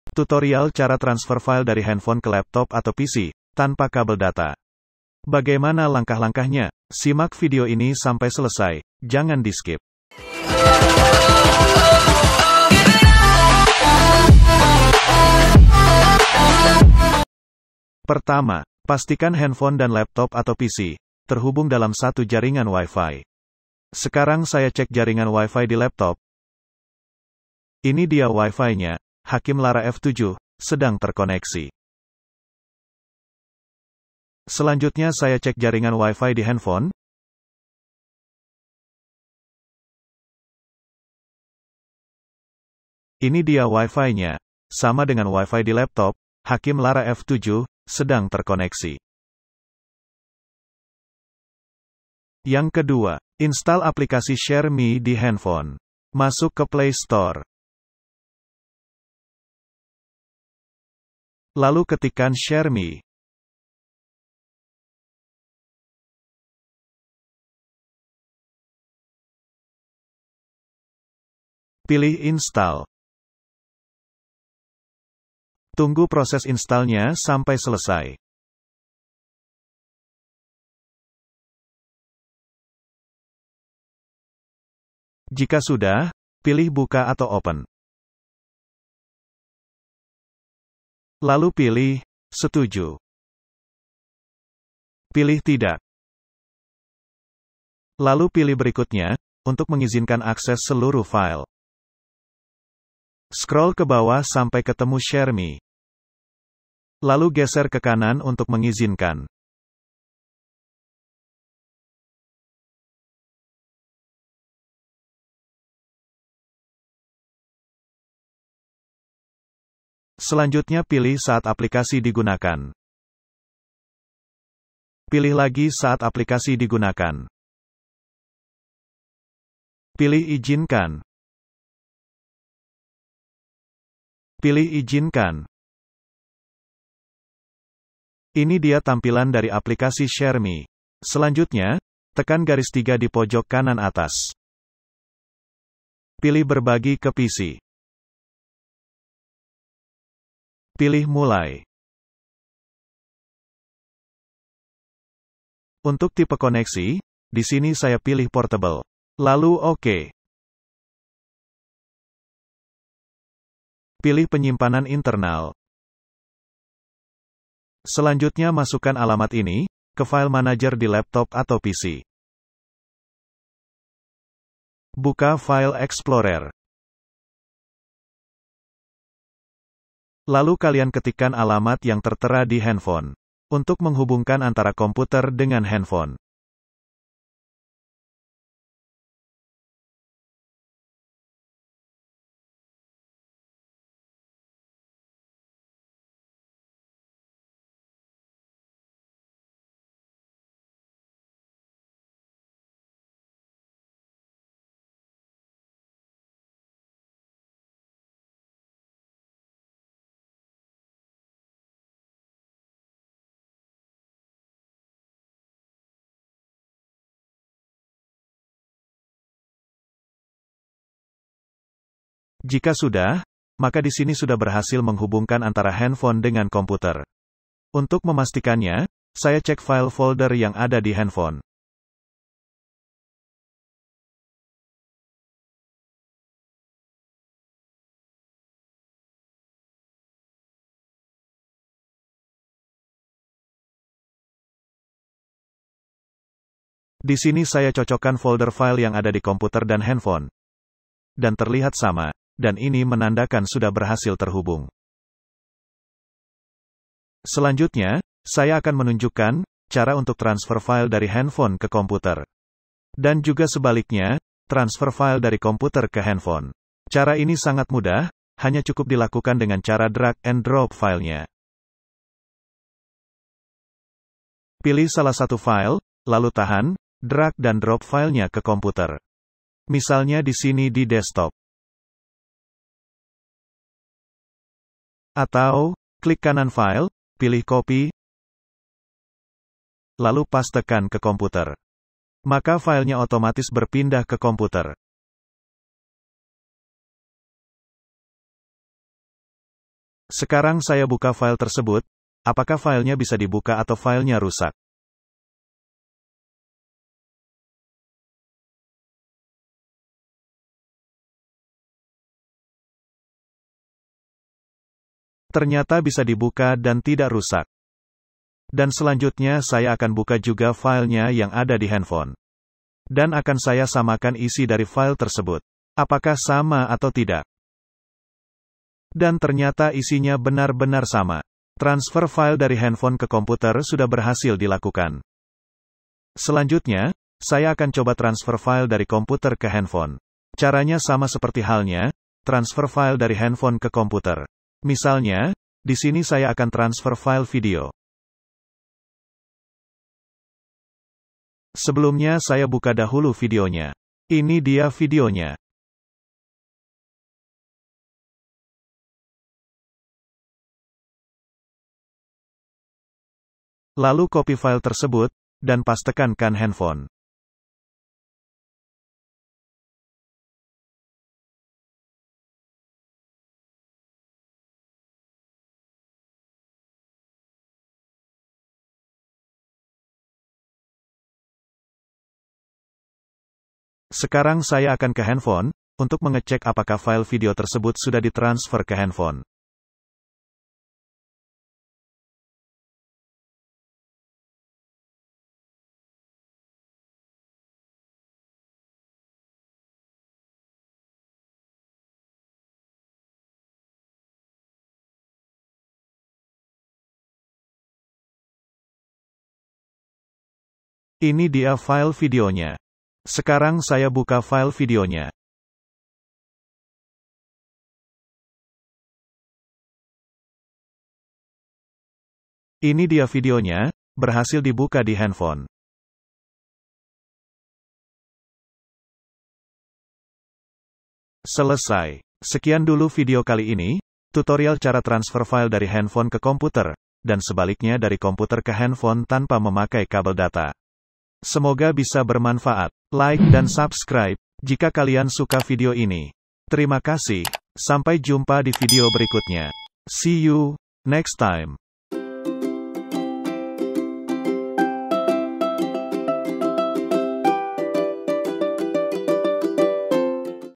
Cara Transfer File dari HP ke PC/Laptop tanpa kabel data - Transfer File dari PC/Laptop ke HP
Cara Transfile File dari HP ke PC/Laptop dan sebaliknya Transfer File dari PC/Laptop ke HP, tanpa menggunakan kabel data.

Ada dua langkah utama yang harus dilakukan :
pertama, pastikan HP dan PC/Laptop berada dalam satu jaringan wifi yang sama
kedua, install aplikasi shareme di HP melalui playstore